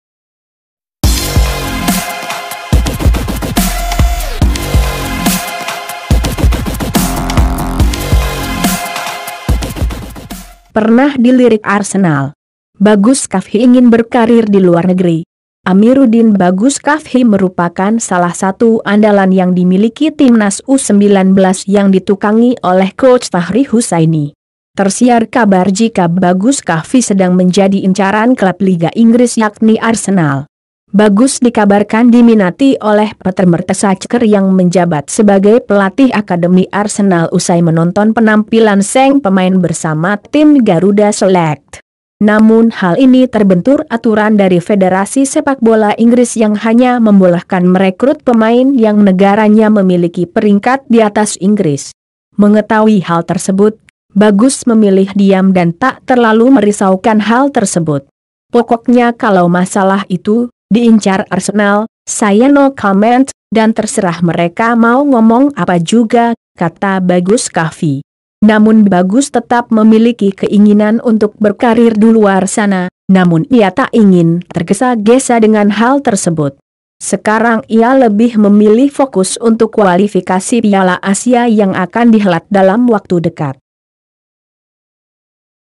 Pernah dilirik Arsenal.Bagus kafhi ingin berkarir diluar negri ?
Amirudin Bagus Kafhi merupakan  salah satu  andalan yang dimiliki timnas u19 yang ditukangi oleh coach  Fakhri Husaini.
Tersiar kabar jika Bagus Kahfi sedang menjadi incaran klub liga inggris yakni arsenal.
Bagus dikabarkan diminati oleh Per Mertesacker yang menjabat sebagai pelatih akademi Arsenal usai menonton penampilan sang pemain bersama tim Garuda Select.
Namun hal ini terbentur aturan dari federasi  sepak bola Inggris yang hanya membolehkan merekrut pemain yang negaranya memiliki peringkat  diatas Inggris.

Mengetahui  hal tersebut,  bagus memilih  diam dan tak terlalu merisaukan hal tersebut.

Pokoknya kalau masalah itu (diincar Arsenal) saya no comment, dan terserah mereka mau ngomong apa juga," kata Bagus Kahfi.

Namun bagus tetap  memiliki  keinginan  untuk  berkarir duluar sana, namun ia tak ingin tergesa-gesa dengan  hal tersebut.  Sekarang  ia lebih memilih  fokus untuk  kualifikasi  Piala Asia yang akan dihelat dalam waktu dekat.